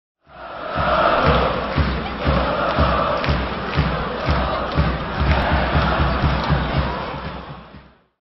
Derby Twójmiasta - 13.04 - Arkowcy dopinguja Drużynę - CracoviaFOTO
⚪️ 🔴Cracovia FOTO 📷

Polub nasze profile i bądź na bieżąco!